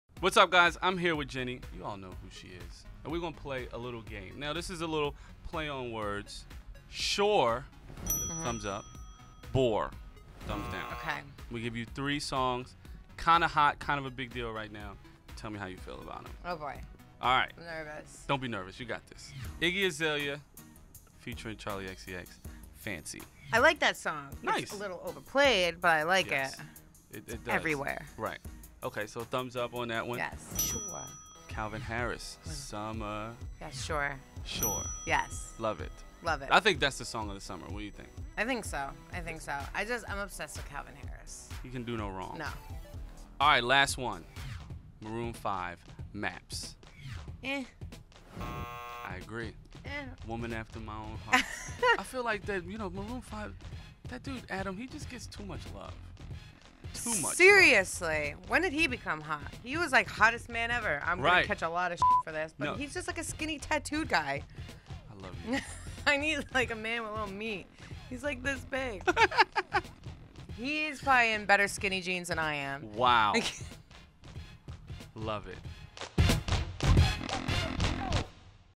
JWoww Rates the Songs of the Summer
Jenni "JWoww" Farley rates the summer's biggest hits in a round of "Shore or Bore"